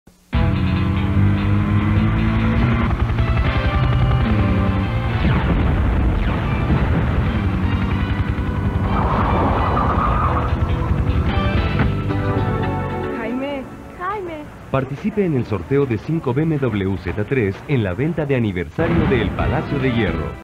Comerciales mexicanos: El Palacio de Hierro 1996
Promoción por una BMW último modelo